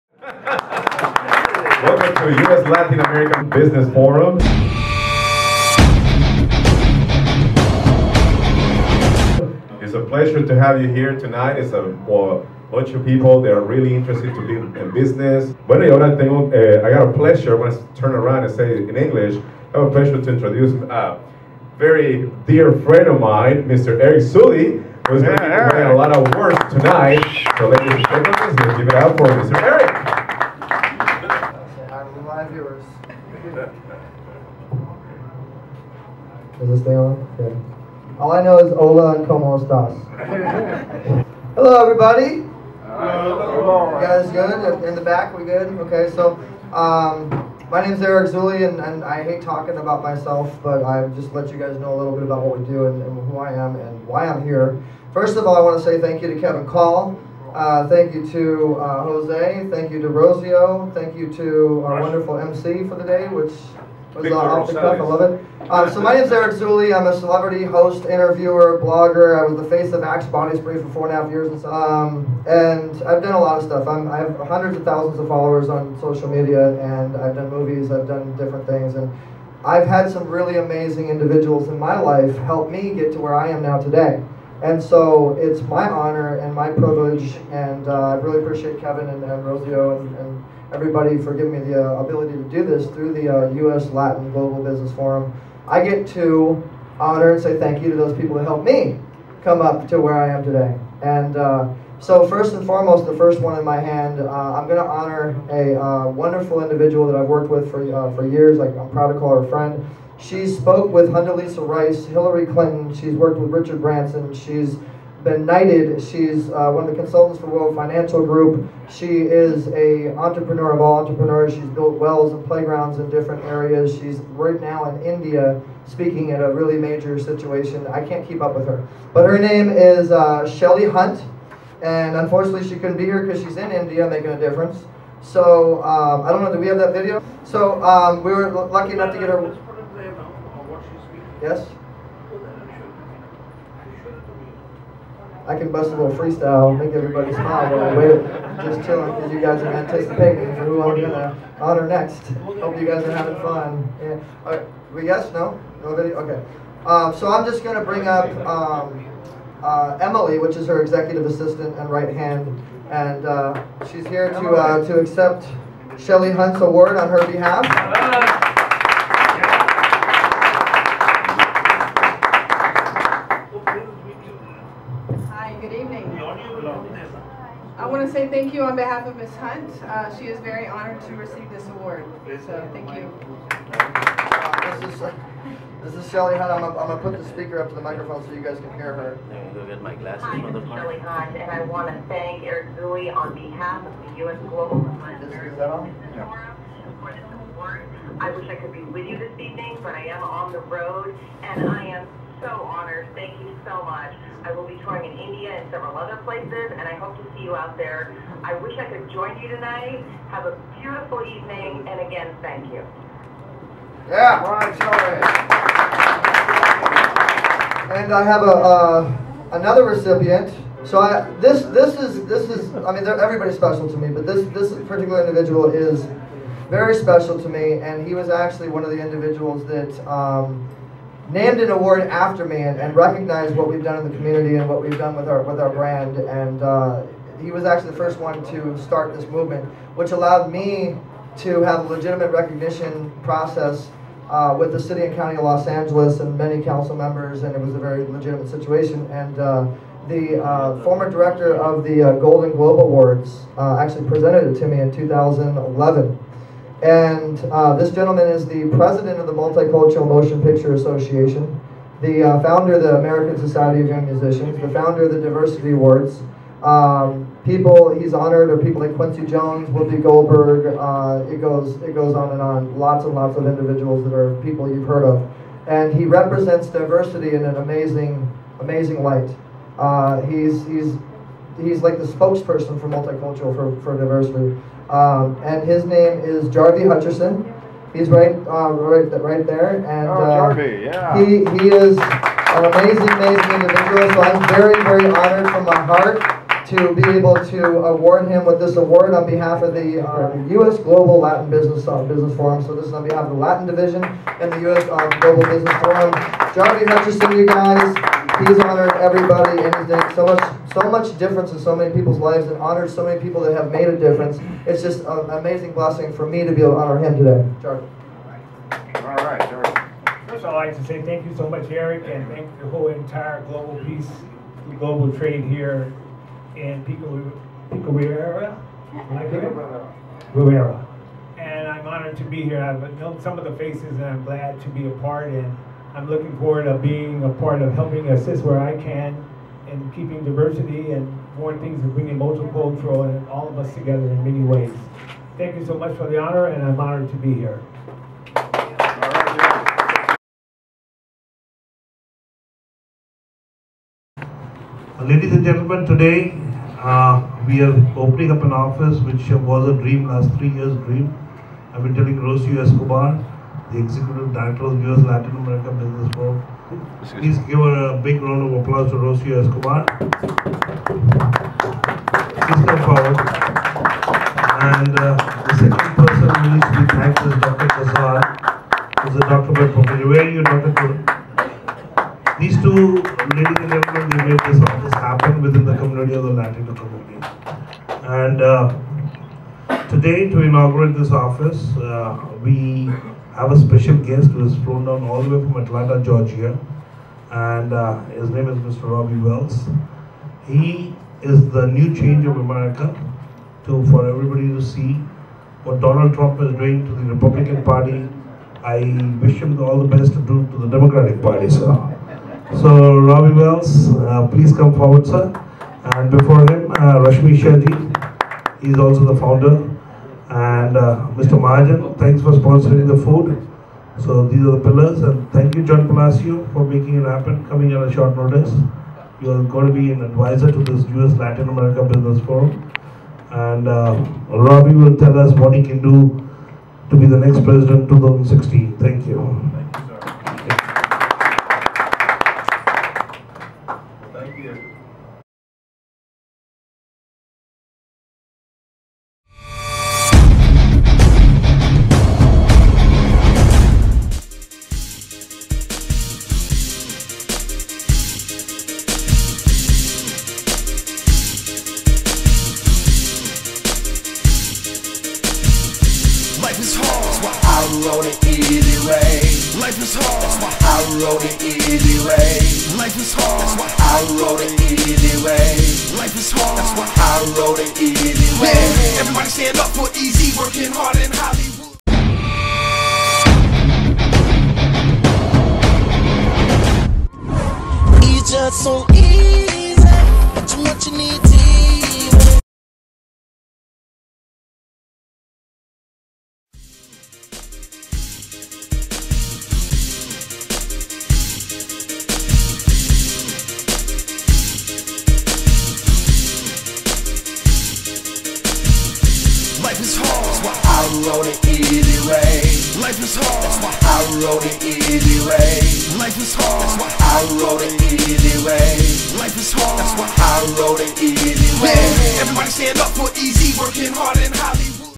Eric Zuley Honors Shellie Hunt & Jarvee Hutcherson US GLOBAL BUSINESS FORUM
Eric Zuley Founder/CEO of EZWAY BROADCASTING, INC honors Shellie Hunt Founder of Women Of Global Change & Jarvee Hutcherson president of the Multicultural Motion Picture Association. This award was on behalf of the US GLOBAL BUSINESS FORUM & US LATIN AMERICA BUSINESS FORUM. This award was in conjuction with 2016 presidential candidate Robby Wells